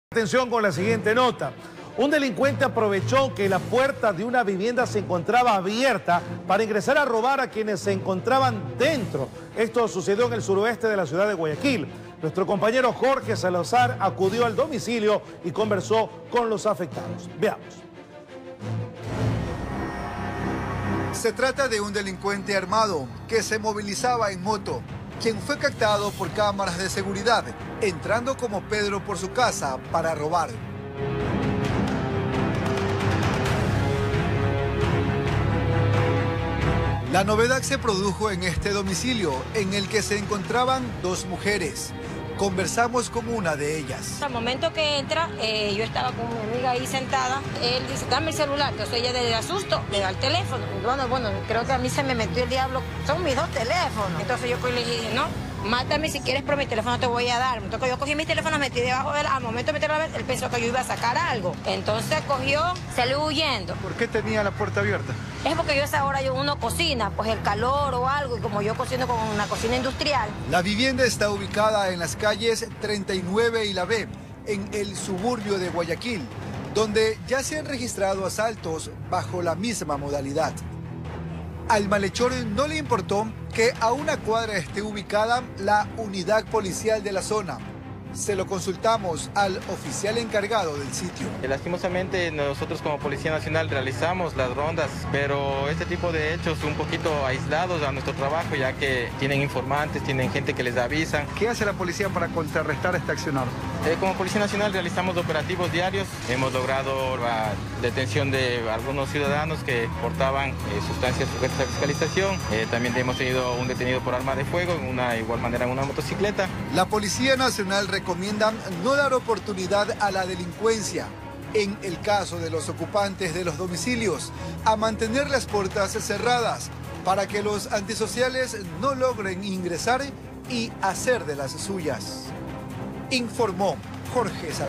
Delincuente roba una casa al Suroeste de la Urbe
La vivienda se encontraba con la puerta abierta.
Cerca de la casa se encuentra una UPC.